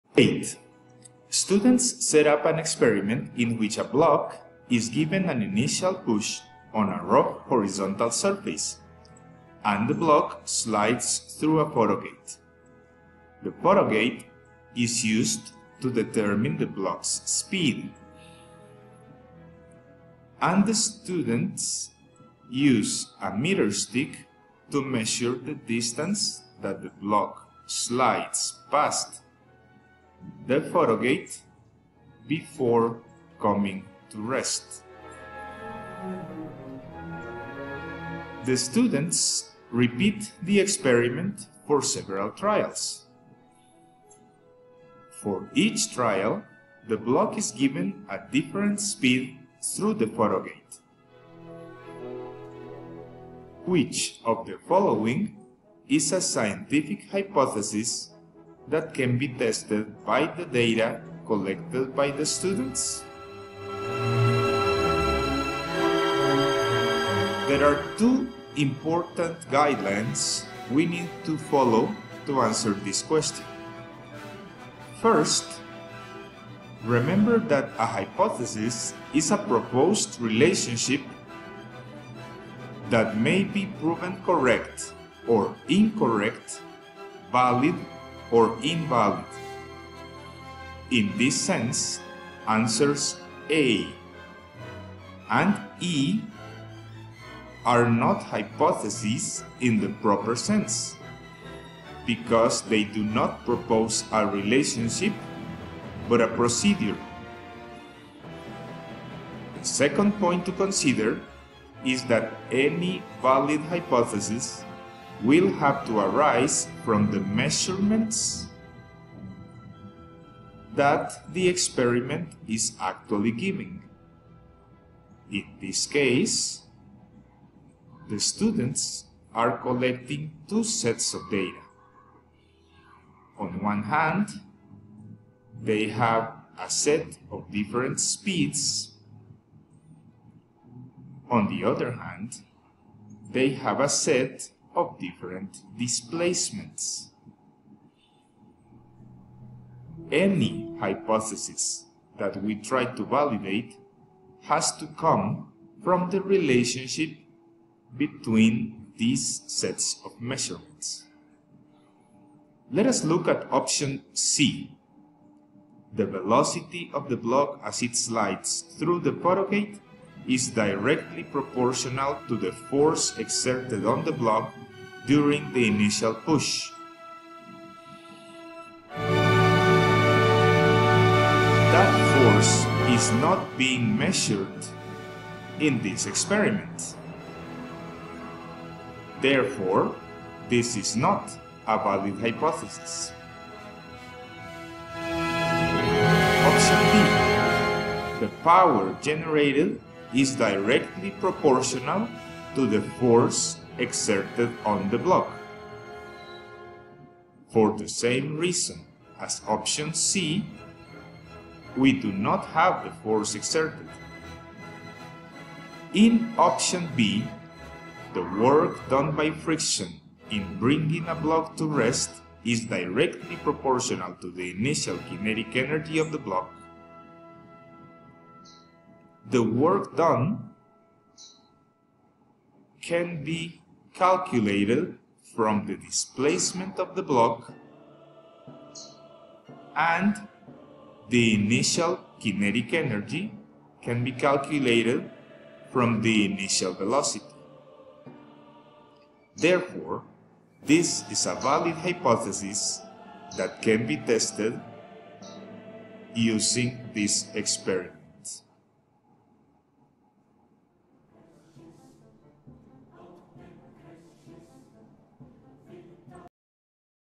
3.1-04 Evaluating the Validity of a Hypothesis from the Work-Kinetic Energy Theorem
The video explains the proper way to determine a valid hypothesis from an experiment in which speed and displacement are measured in order to test the work-kinetic energy theorem. We must pay close attention to the experimental setup.